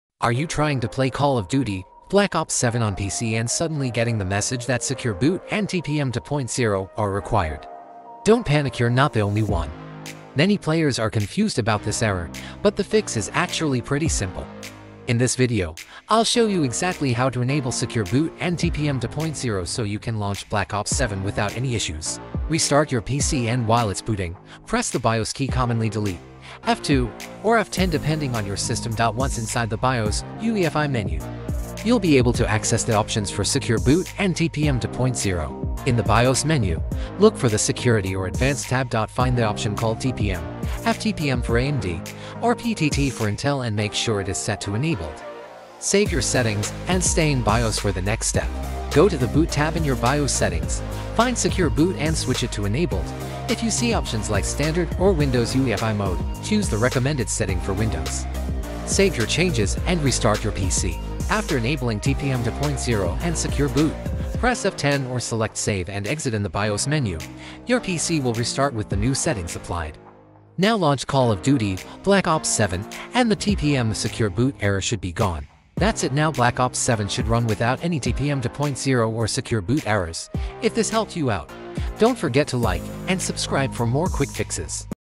Fix Call of Duty Black Ops 7 Secure Boot & TPM 2.0 Error | How to Enable TPM 2.0 & Secure Boot on PC
Are you getting the Secure Boot & TPM 2.0 Required error while trying to play Call of Duty: Black Ops 7 on PC? Don’t worry—you’re not alone. This is a common problem, and the fix is very simple.

In this tutorial, I’ll show you step by step how to enable TPM 2.0 and Secure Boot in your BIOS/UEFI settings. These are required by Windows 11 and some new games like Black Ops 7 to run properly.

What you’ll learn in this video:

How to enter BIOS/UEFI on your PC

How to enable TPM 2.0 (Intel PTT / AMD fTPM)

How to enable Secure Boot mode

How to save settings and fix the error

Follow this guide and you’ll be able to launch Black Ops 7 without any TPM or Secure Boot issues.